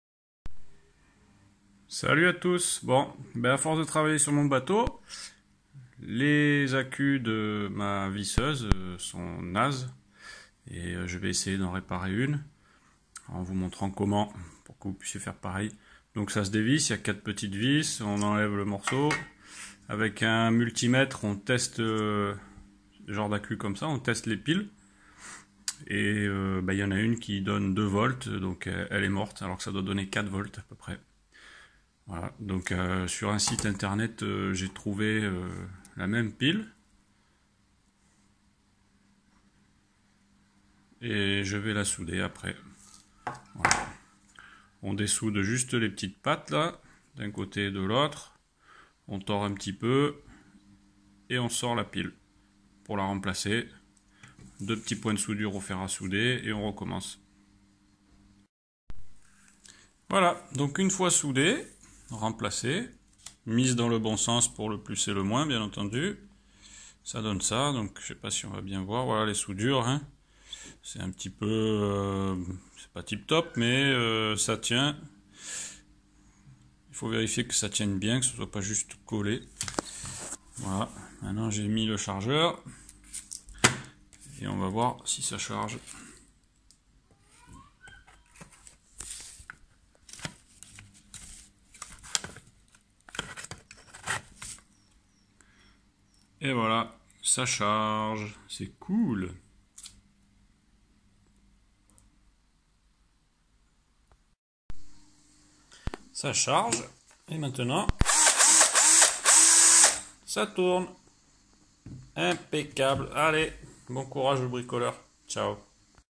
Batterie visseuse Makita réparation
Une petite vidéo montrant comment réparer une batterie de visseuse Makita a moindre frais..